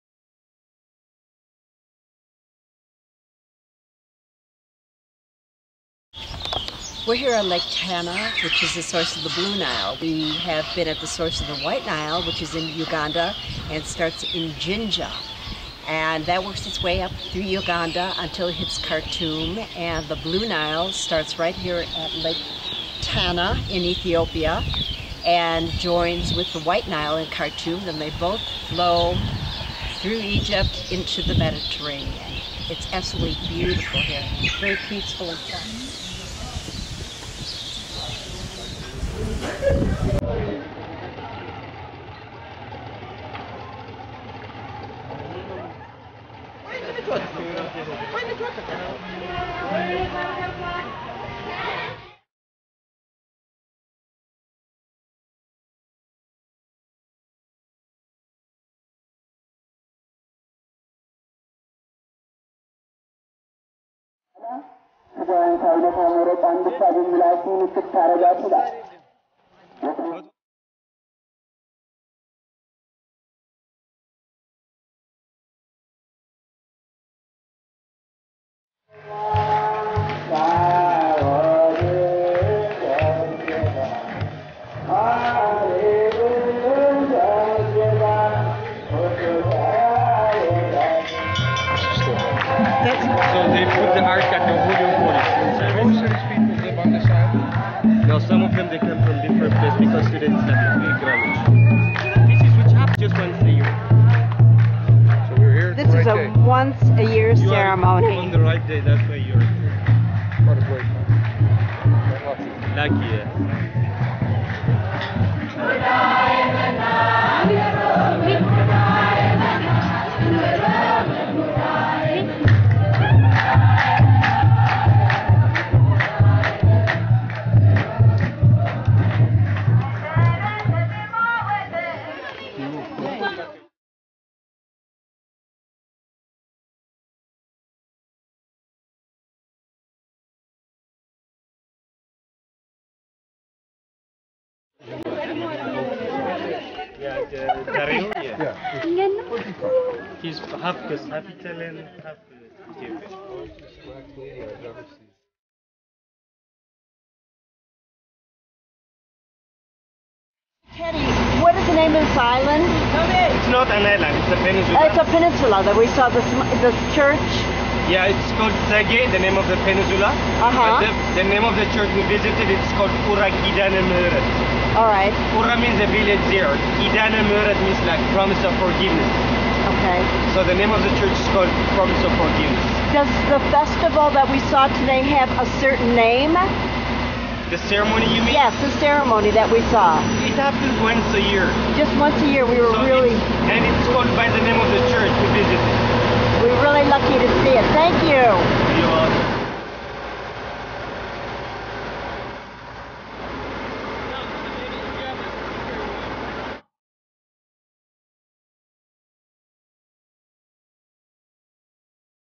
North Ethiopia: Bahirdar, Lake Tana & A Festival
Boating over Lake Tana to Ura Kidane Monastery, we were fortunate to see a once-a-year celebration.  A mixture of African and devout Orthodox Ethiopian Christianity, it was spine-tingling.  Ura Kidane is the Zege Peninsula's most famous monastery and has beautifully painted depictions of Jesus, Mary and every other saint.  Primitive, bold and thoroughly engrossing...